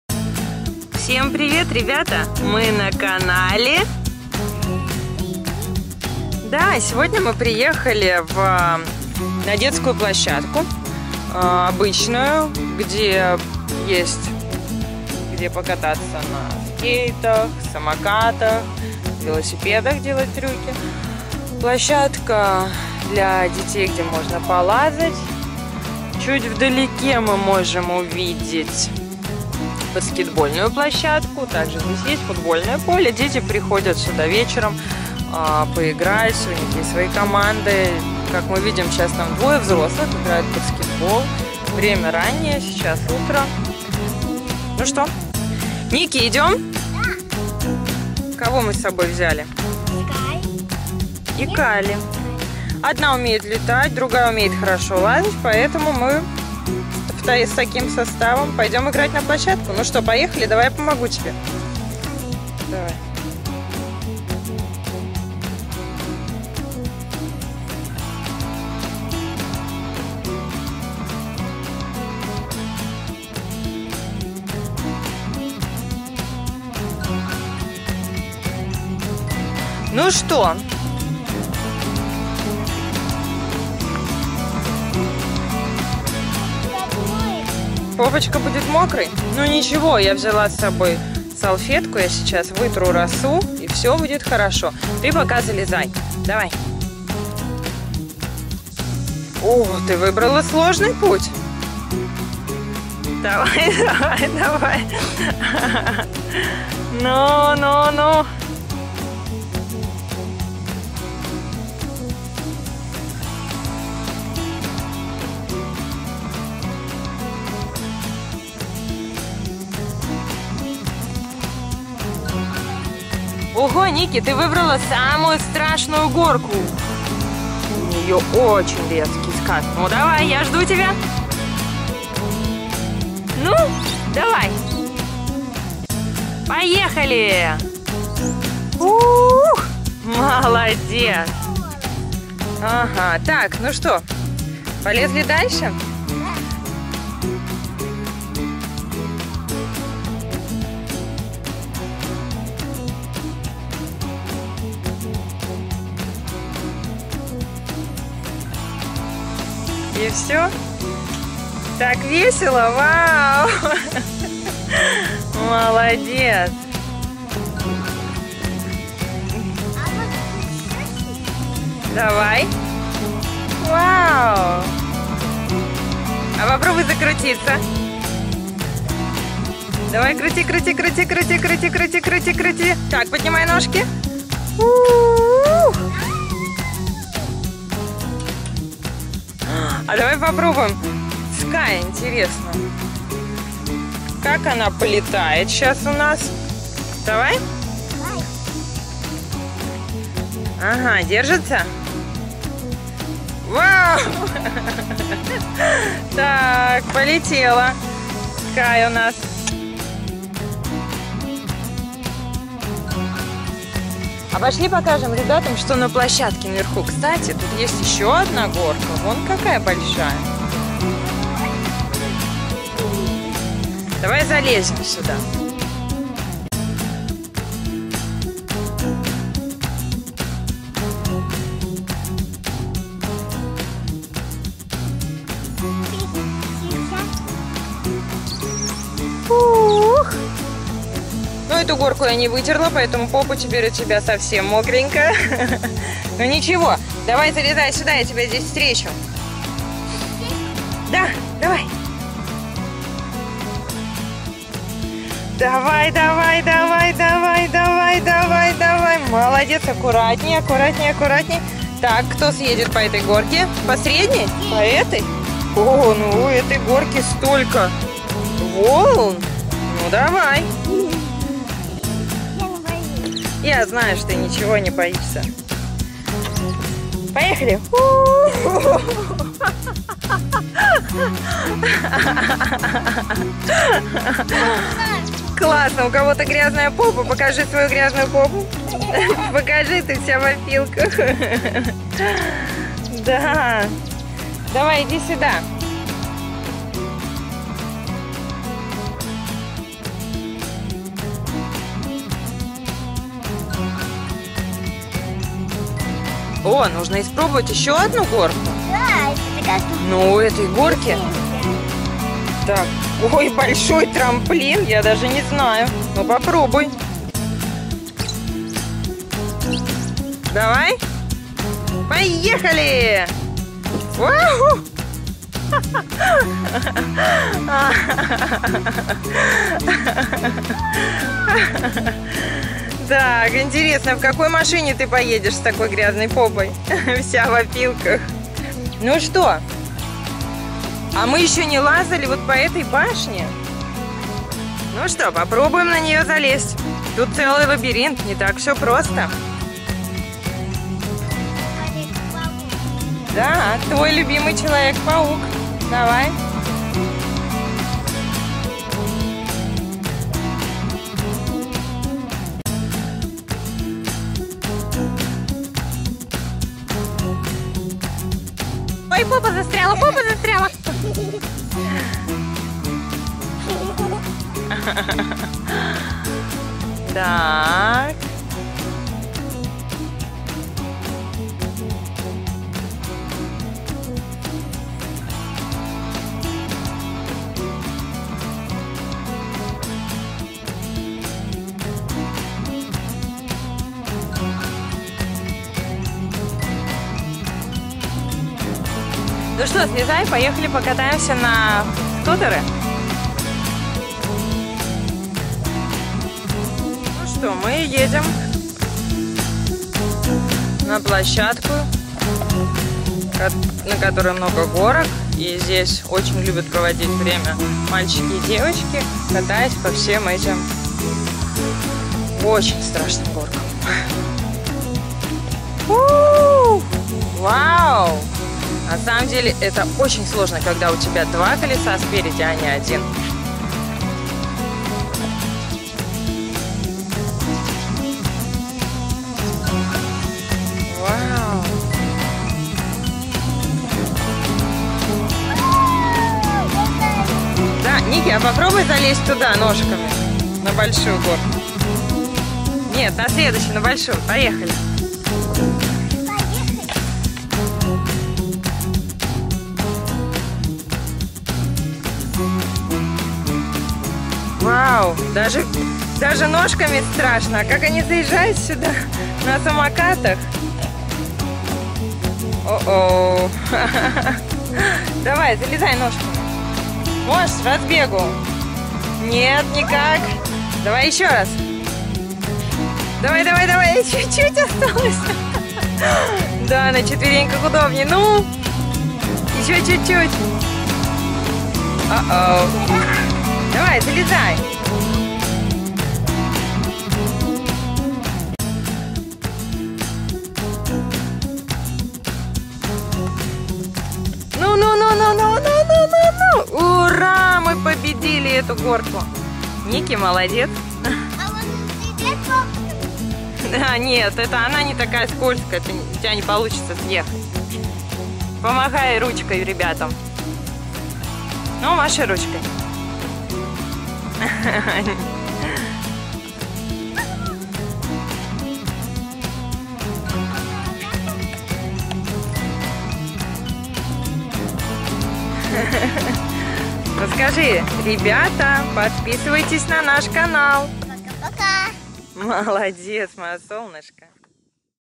ЩЕНЯЧИЙ ПАТРУЛЬ и НИКА на детской площадке
Ника, ее мама а так же щенячий патруль приехали на детскую площадку что бы опробовать все горки и качели  Нужно испробовать все горки, полазить по веревкам , прокатить Скай на качелях.